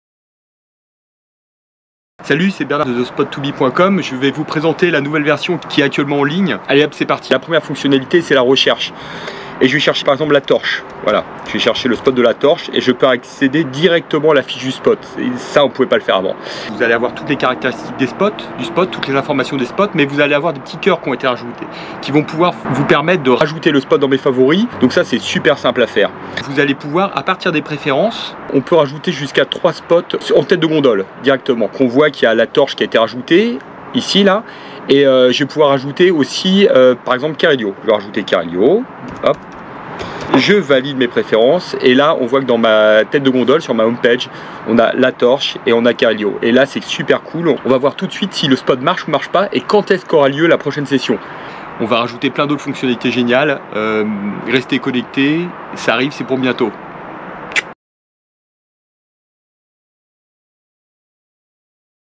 Thespot2be.com, nouvelle version
What's up ? En 1 minute, voici les grands ajouts de cette nouvelle version !